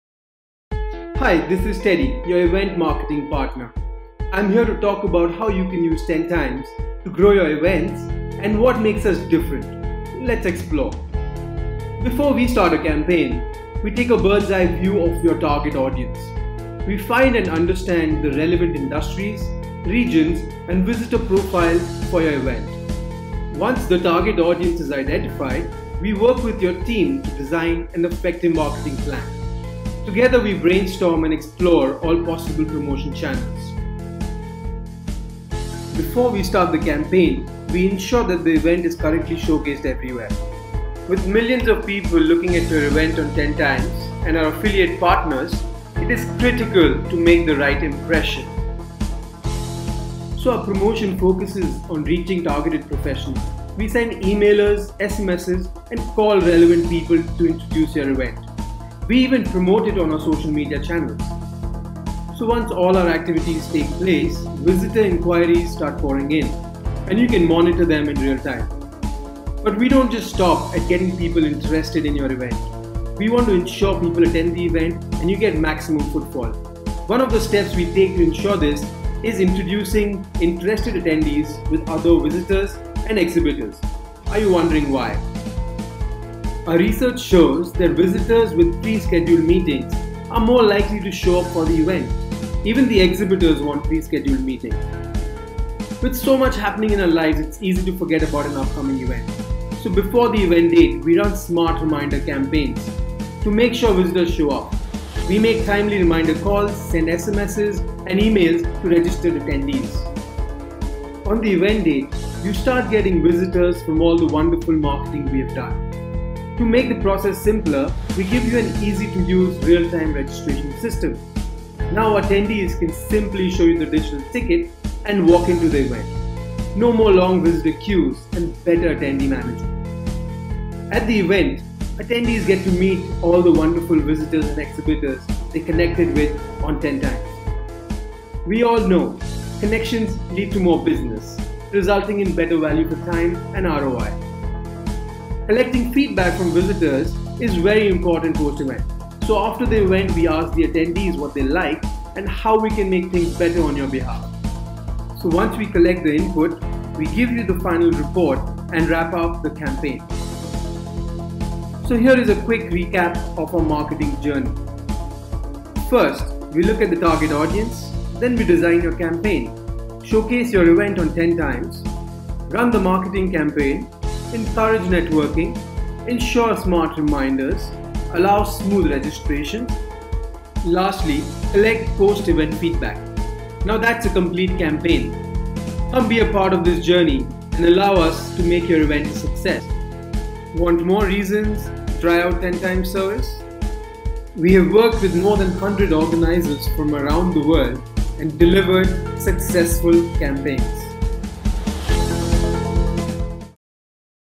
10Times: Your event marketing partner
10Times is a platform that helps you reach your target event audience, generate interest for your events and brings your relevant visitor traffic. To do this, we use technology+behavioural analysis to piece the puzzle together.